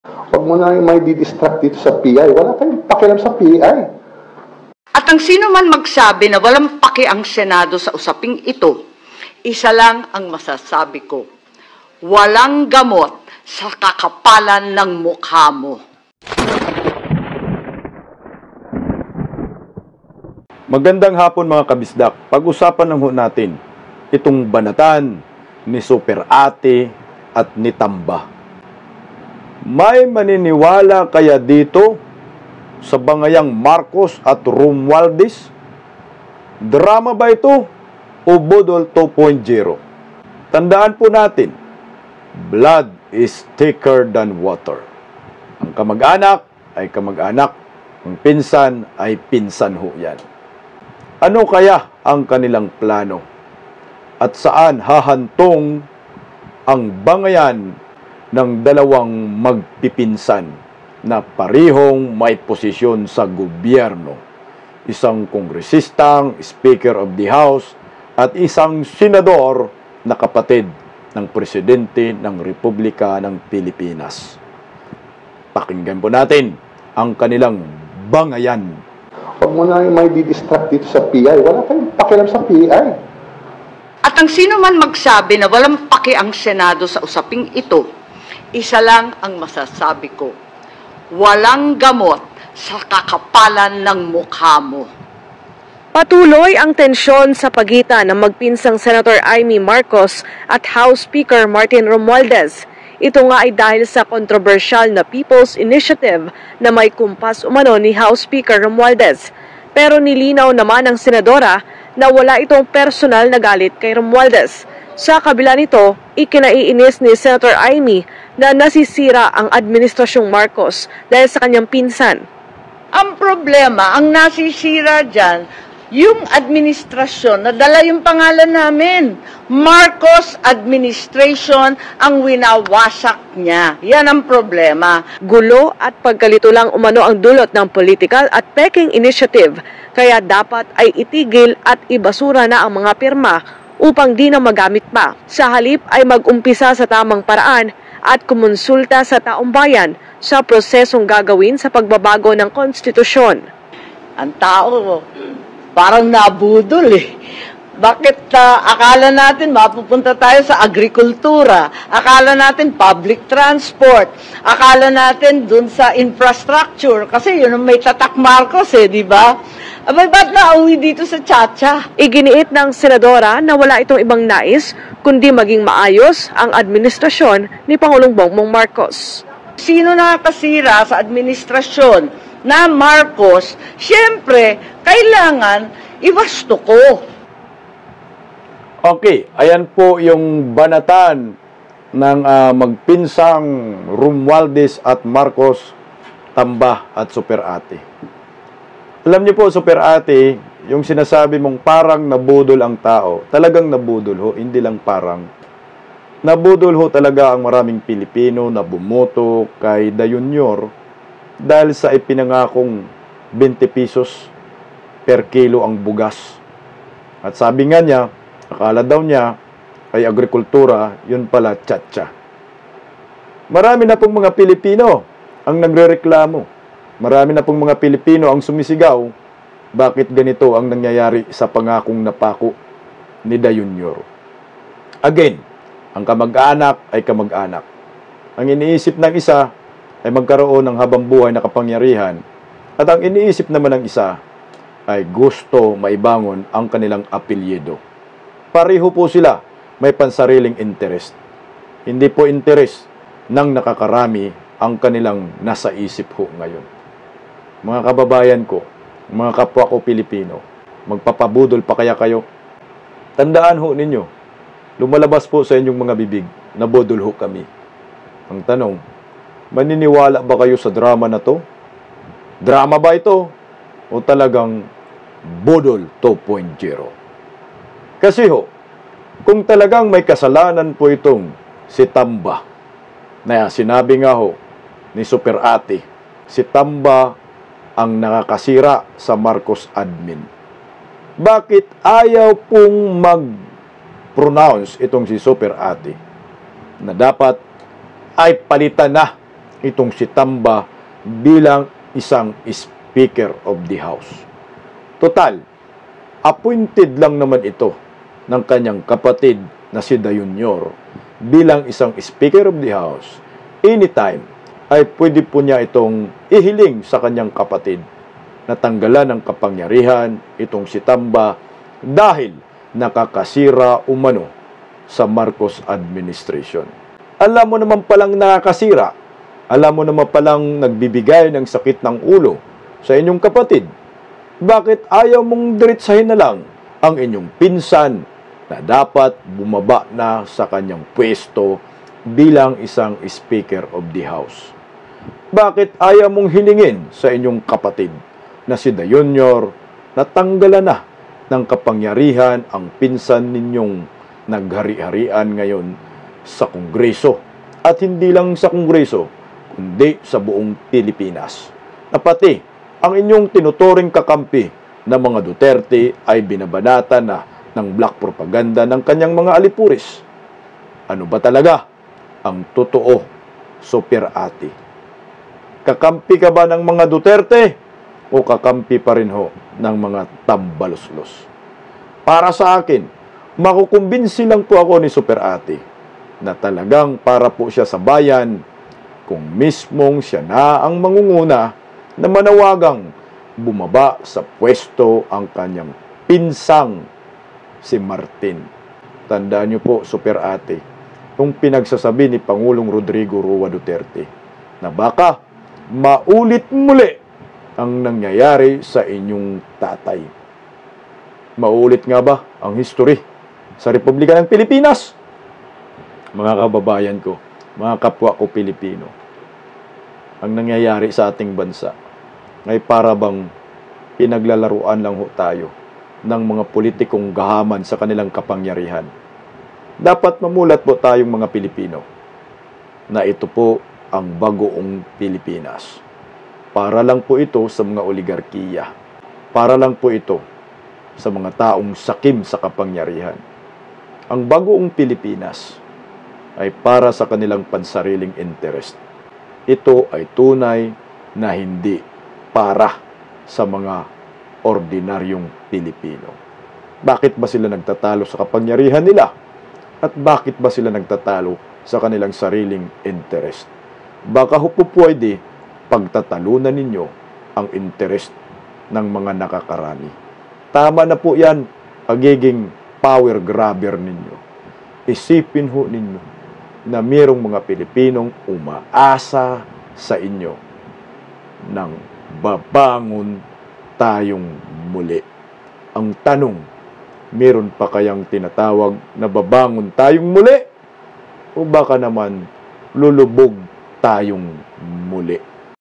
BANGAYAN NG MARCOS AT ROMUALDEZ DRAMA O BUDOL 2.0?
Super Ate at Tamba tumindi ang bangayan tungkol sa P I? Maniniwala ba kayo dito? Drama o Budol?